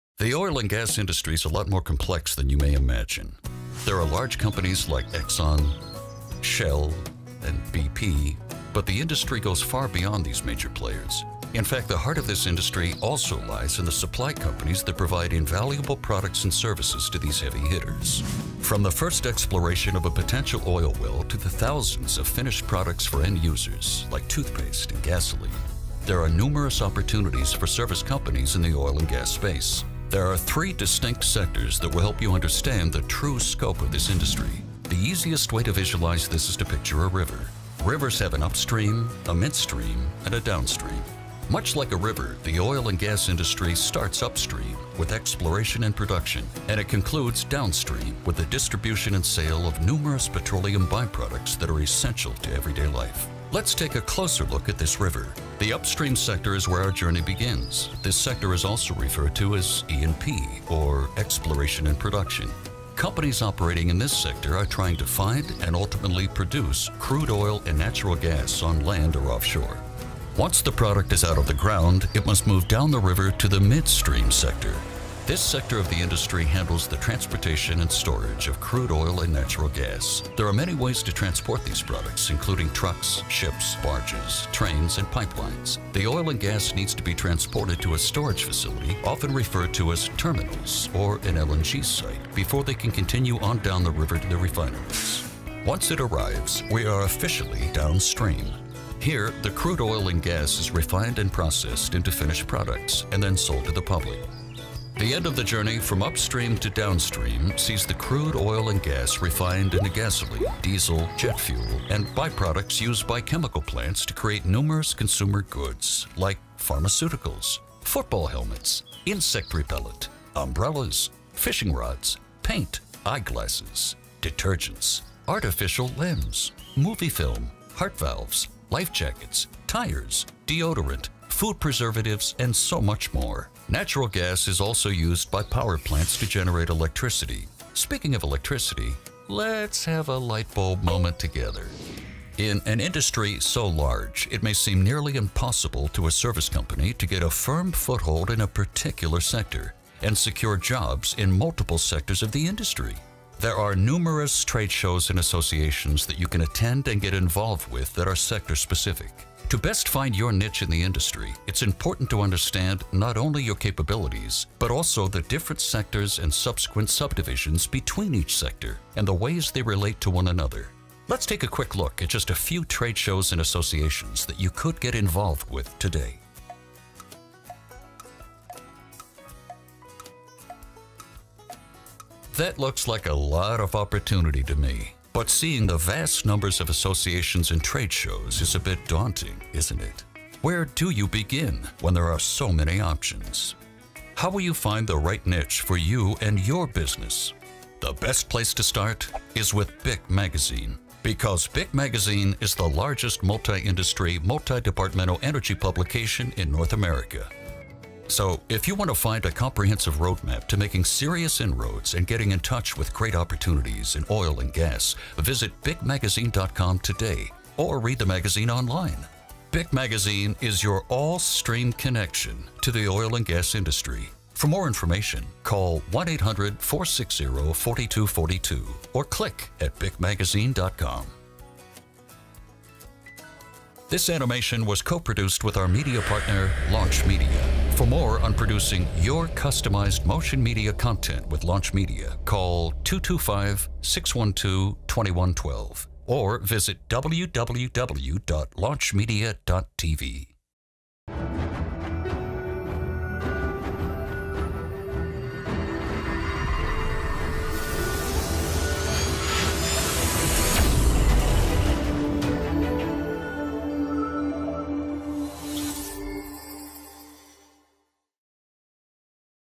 Understanding the Oil and Gas Value Chain: Upstream, Midstream, and Downstream
Upstream: exploration & production. Midstream: transportation & storage. Downstream: refining, distribution, and marketing of oil and gas products.